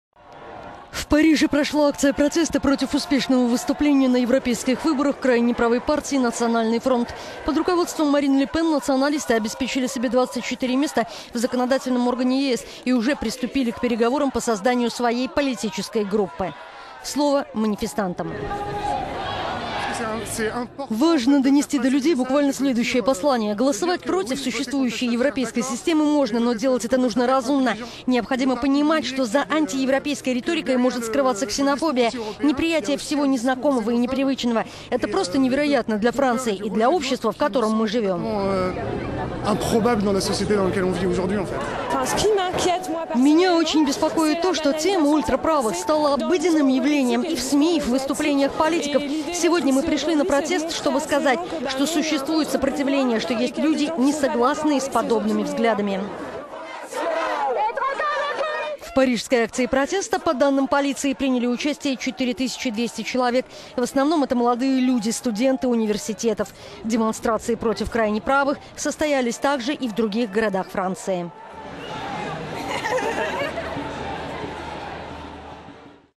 Во Франции прошли протесты против Национального фронта
В Париже прошла акция протеста против успешного выступления на европейских выборах крайне правой партии Национальный фронт. Под руководством Марин Ле Пен националисты обеспечили себе 24 места в законодательном органе ЕС и уже приступили к переговорам по созданию своей политической группы.

Слово манифестантам:

"Важно донести до людей буквально следующее послание: голосовать против существующей европейской системы можно, но делать это нужно разумно. Необходимо понимать, что за  антиевропейской...